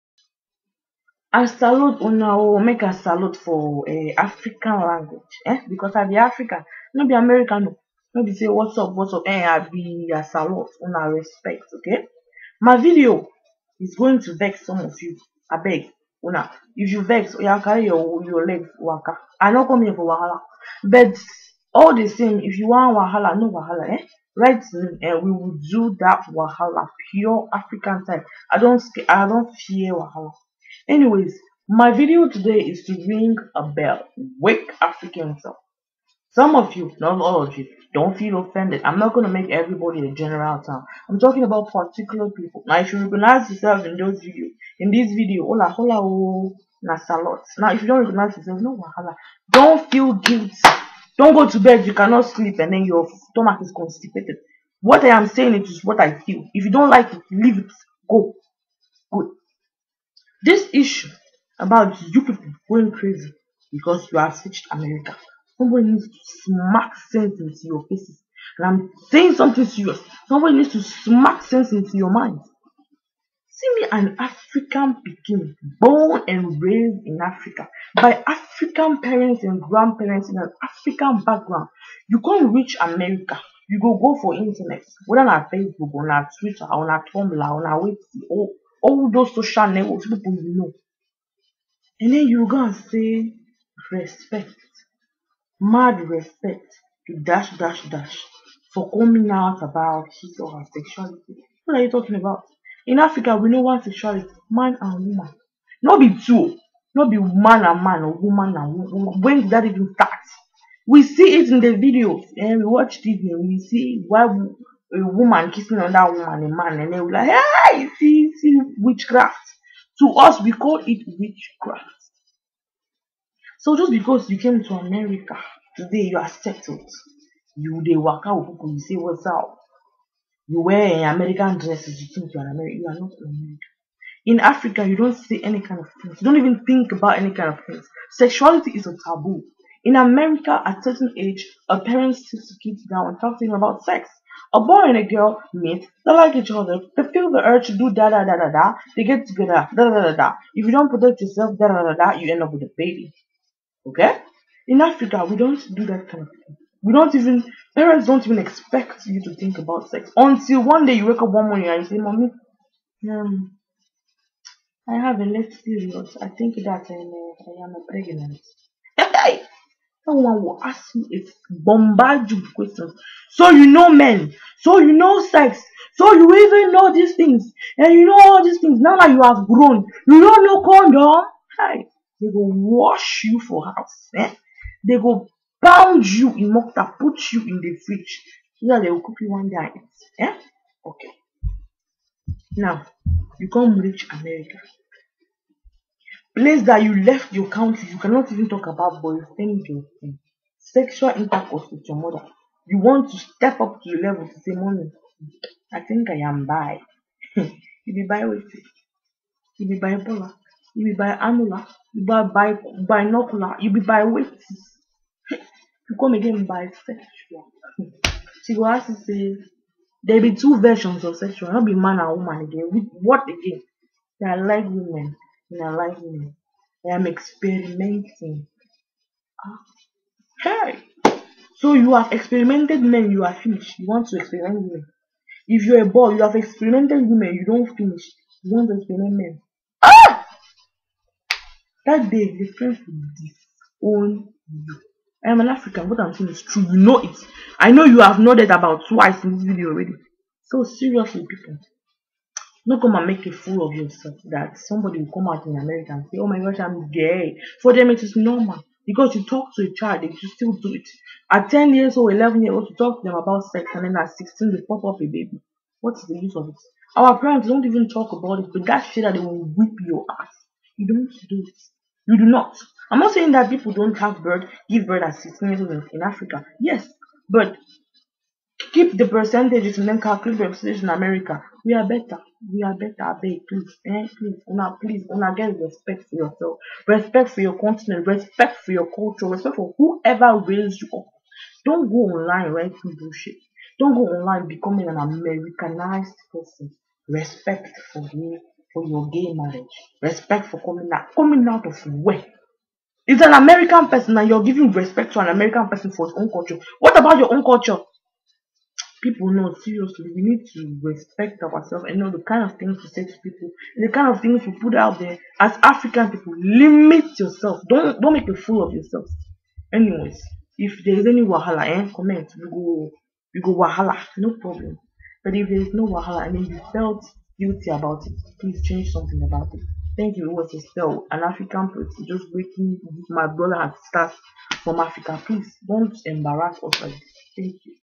AFRICAN WAKE UP CALL
SO THIS IS A WAKE UP CALL TO MY FELLOW AFRICANS..IF YOU FEEL OFFENDED IT`S LIFE AND IF YOU HEAR SOME SENSE IN THIS THEN GOOD..ANYWAYS I SPOKE MY MIND NOT TO PLEASE ANYONE ...